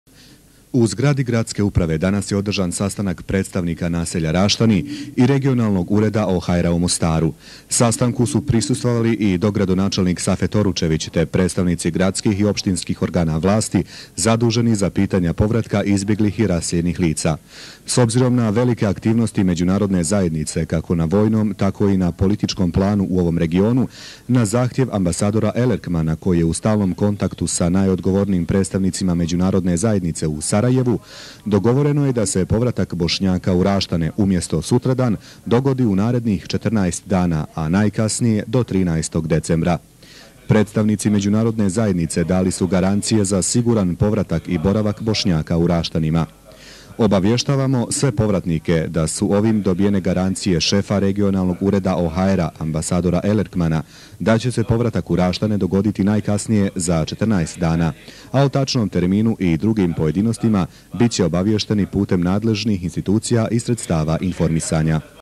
1999. Sastanak u Gradskoj upravi o problemu povratka u Raštane
Zapratite kanal Mostar Memorial & Museum, tu smo da podsjetimo i sačuvamo od zaborava sve ono što su mostarci i Mostar proživjeli.